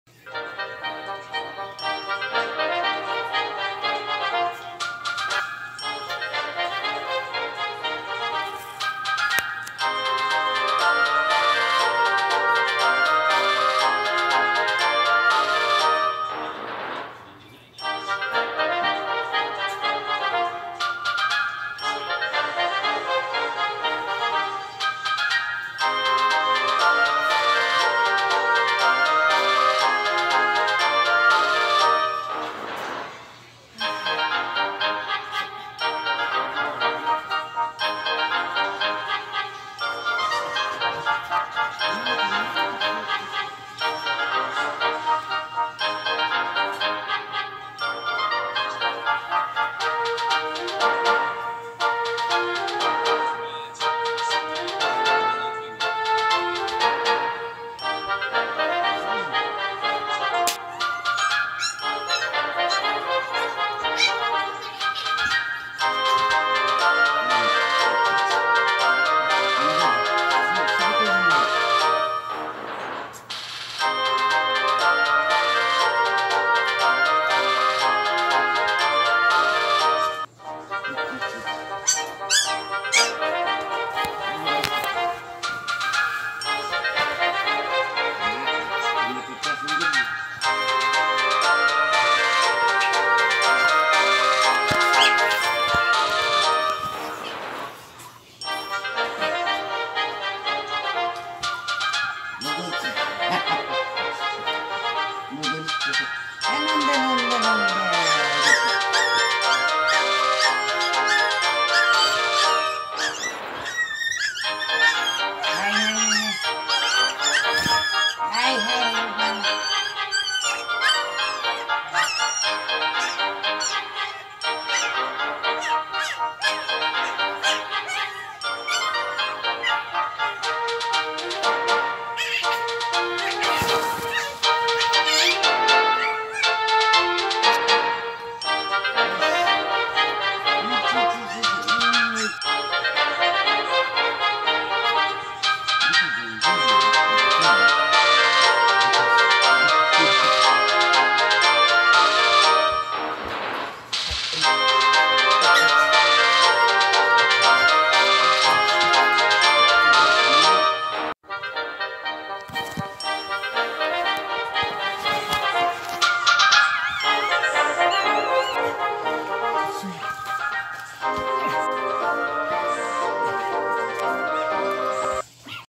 自分より小さな赤ちゃんトイプードルがこわいシーズーの赤ちゃん
シーズーのすももちゃん２ヶ月はちびっこトイプードルに興味津々
でも無邪気にかけよるちびっこが怖くてたまりません(ﾉ≧▽≦)ﾉ#シーズー#shihtzu#トイプードル#兄弟#姉妹#トイプー#赤ちゃん#子犬#love#toy poodle dogs#puppy#pup#voice#鳴き声#大好き#友達#遊ぶ